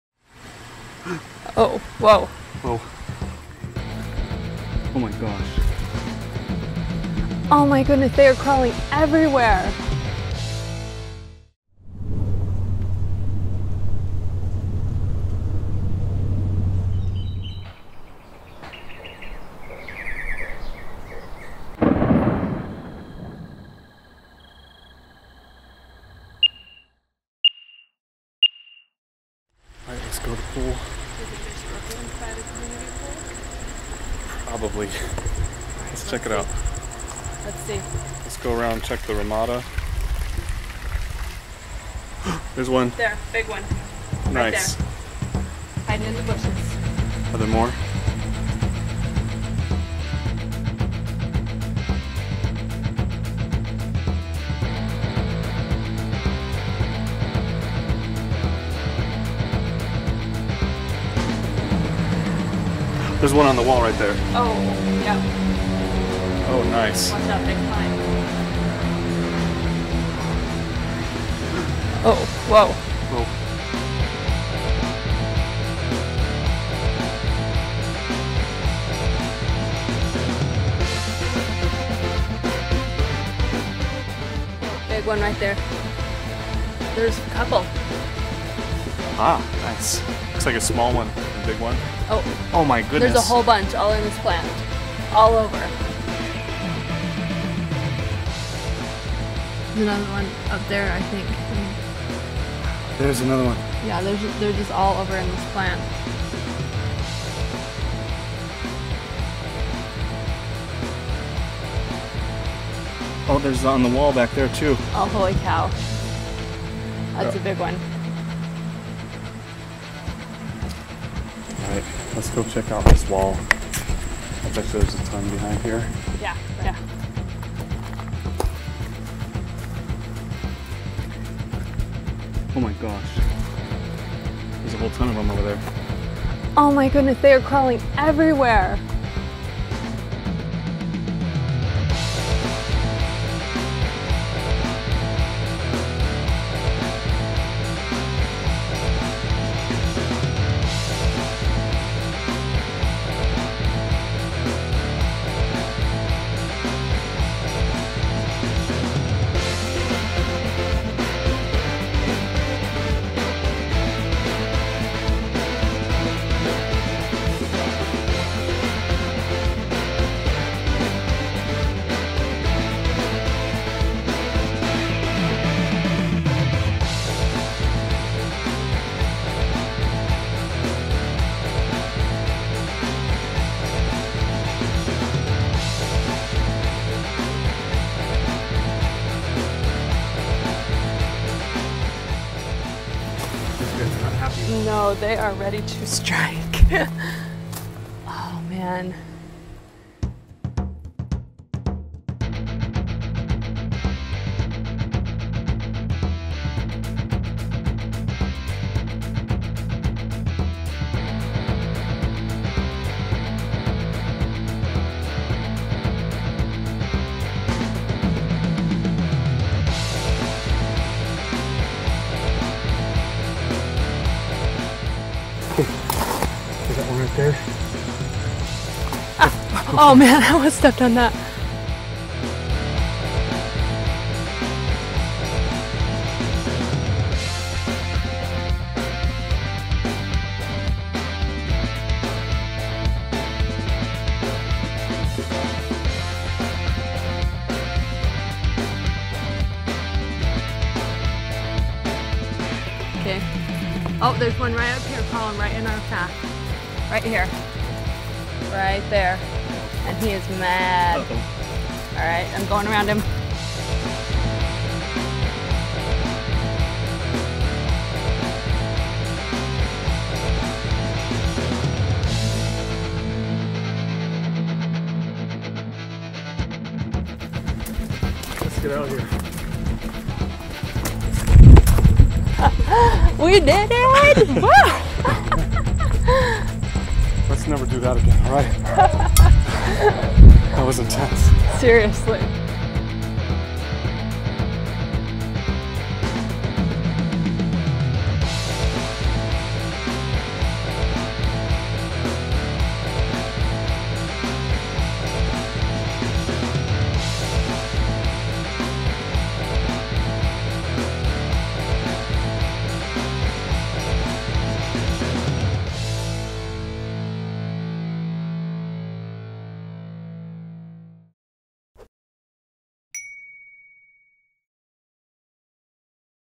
20 Deadly Scorpions - Hunting in Black Light Phoenix Arizona Video
Bark scorpions are everywhere! We found 20 of the most venomous scorpions in North America near a community swimming pool in Gilbert, Arizona (near Phoenix). Using a black light and the best scorpion spray available, we went on a hunt to find and exterminate them. Our goal is to keep our kids safe.

Keep your family and kids safe by using proper extermination techniques. Call an exterminator to spray for bugs (the scorpions' food) and use the equipment below to kill them individually. Do this 1-2 times per week and you should be able to get them under control.

51 LED Black Light
Or, try the ultimate UV Beast flashlight!

Terro Scorpion Killer Aerosol Spray (the best! Used in the video)

UV Safety Glasses

The inexpensive action camera we used to film this video